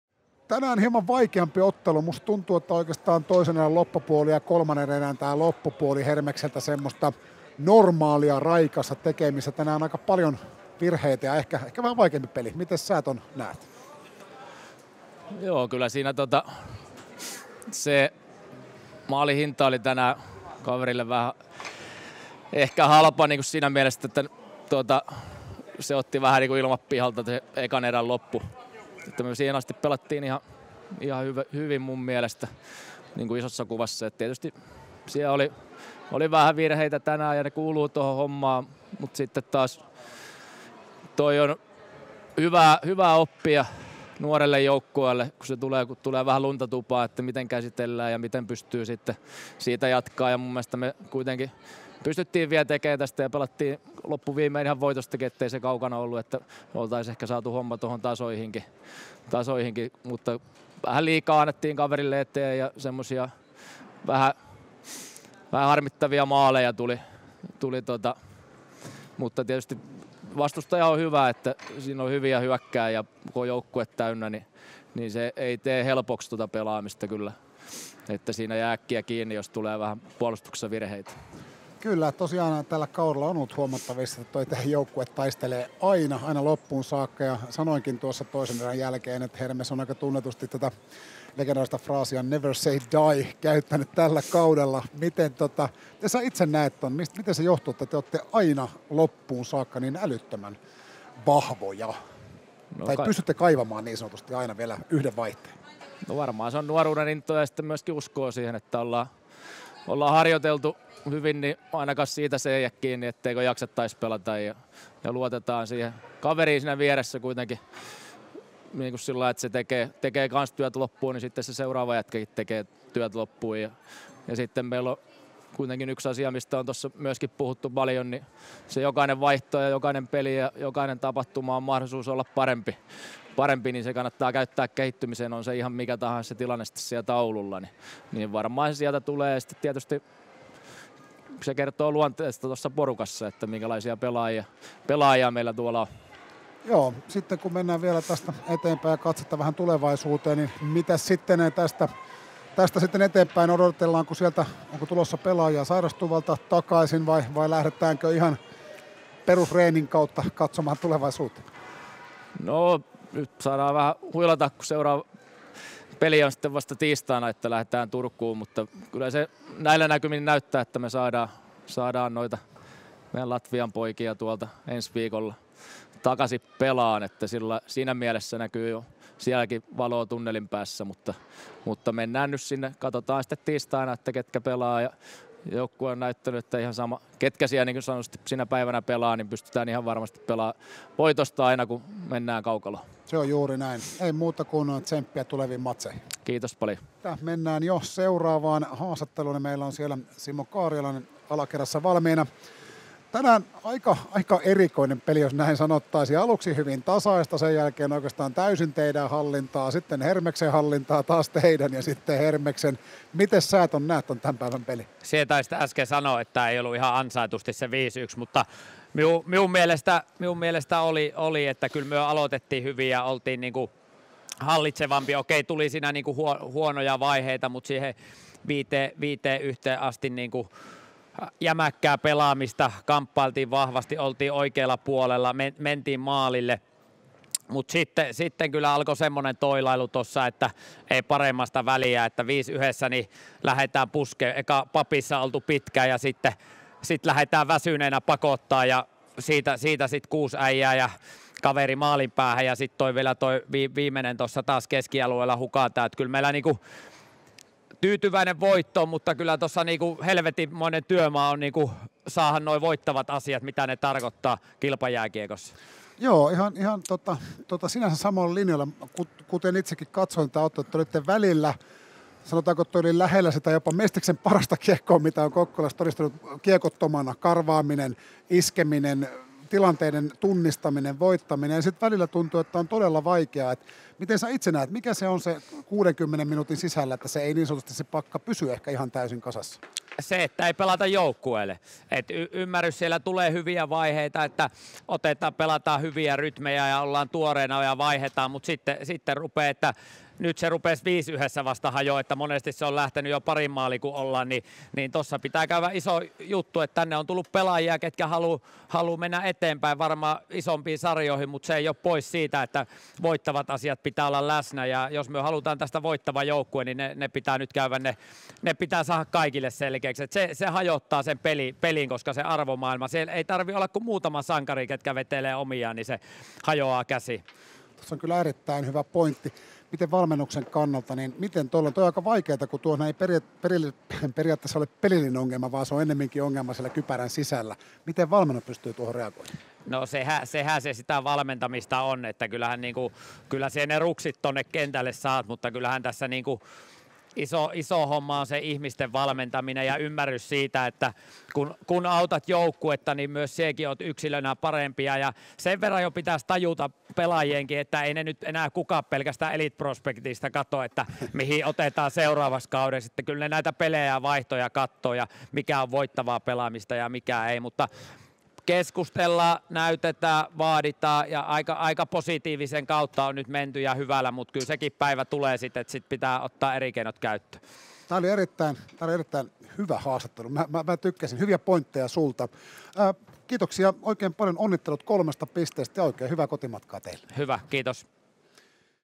Hermes - JoKP pe 26.11.2022 | Hikihaastattelut
Ottelun jälkeen haastattelussa JoKP:n päävalmentaja Simo Karjalainen ja Hermeksen päävalmentaja Tuukka Poikonen. Toimittajana Niko-Joonas Tuominen.

Kuva: Cmore
Editointi: John Sundvik